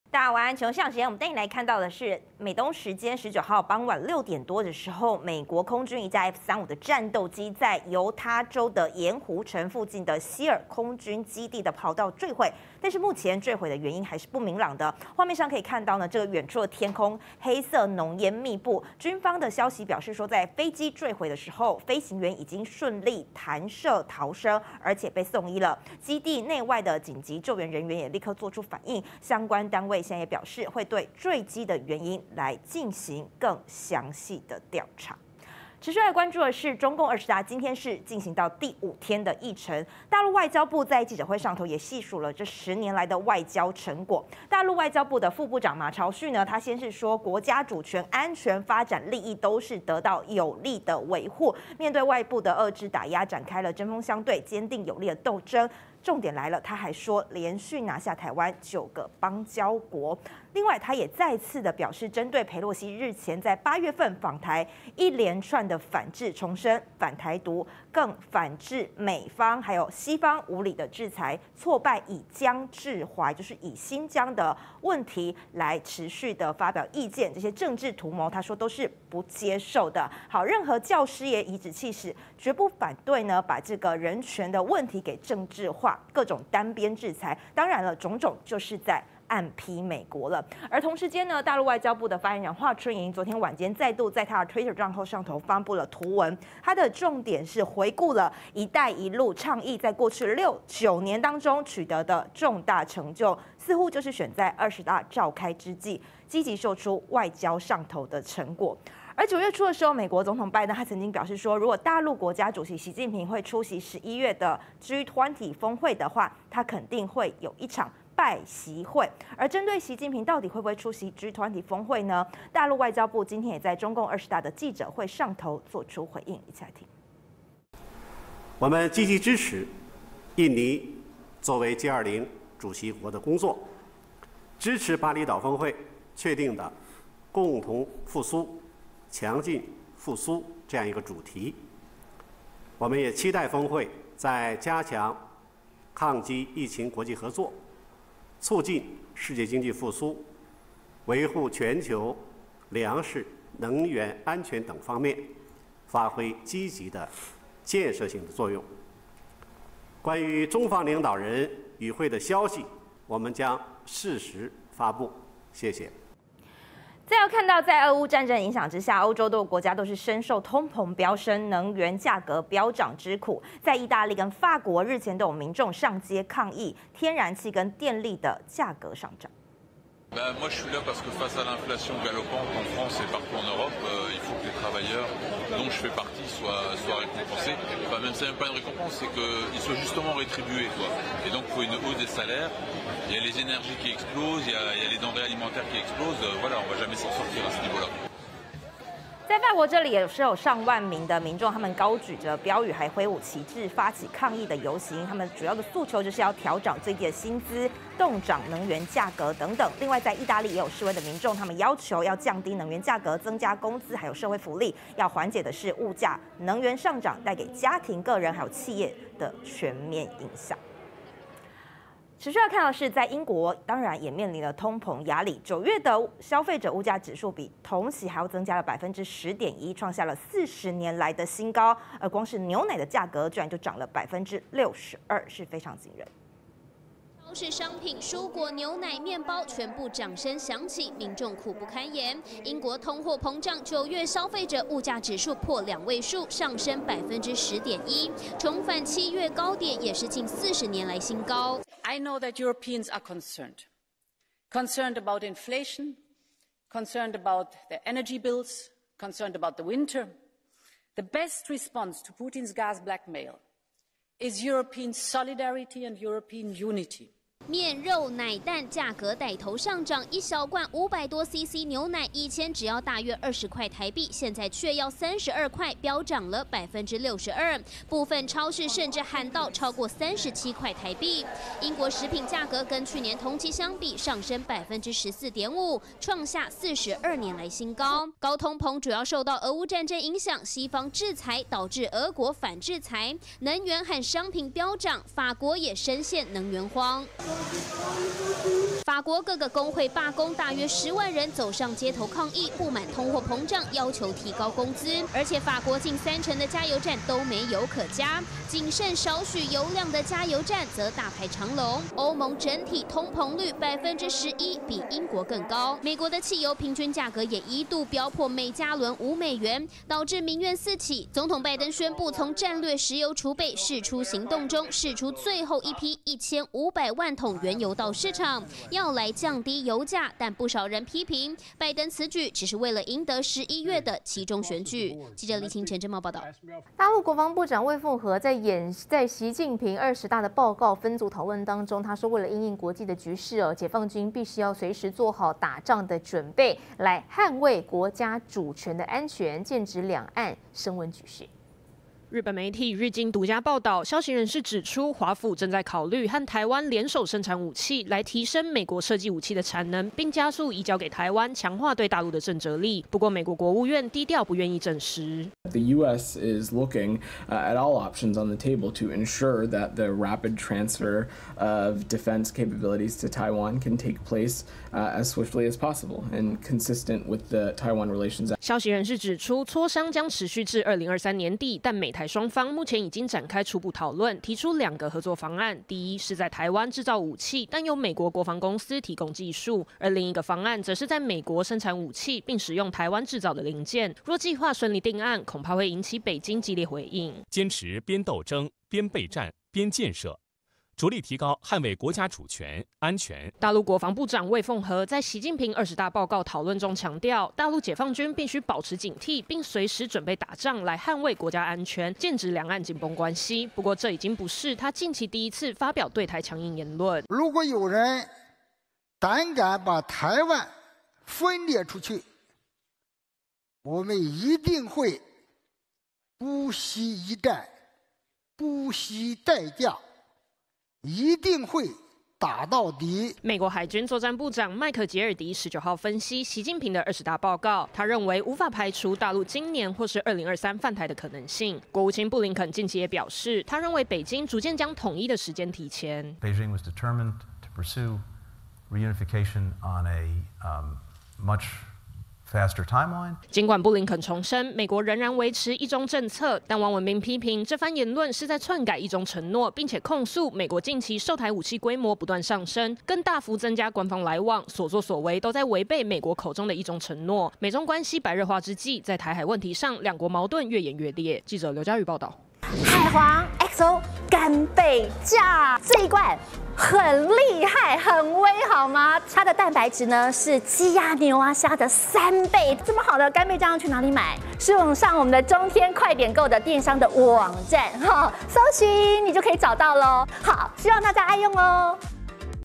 【每日必看】中共二十大 陸外交部:連拿下台灣9個邦交國｜G20將有拜習會? 陸外交部:中方會適時發布消息 @中天新聞CtiNews 20221020
00:00 美軍F-35墜毀基地跑道! 狂冒濃煙 飛行員緊急彈射
00:36 中共二十大 陸外交部:連拿下台灣9個邦交國
01:33 二十大談外交 馬朝旭:堅決反對任何台獨行徑
01:40 中共二十大 華春瑩推特PO出一帶一路成就
01:50 G20將有拜習會? 陸外交部:中方會適時發布消息
02:46 薪資凍漲 能源價格飆 義法民眾上街抗議
03:42 倫敦蔬果9月通膨率10.1％ 創40年新高
05:37 日經:美台擬聯手生產武器抗陸 美低調未證實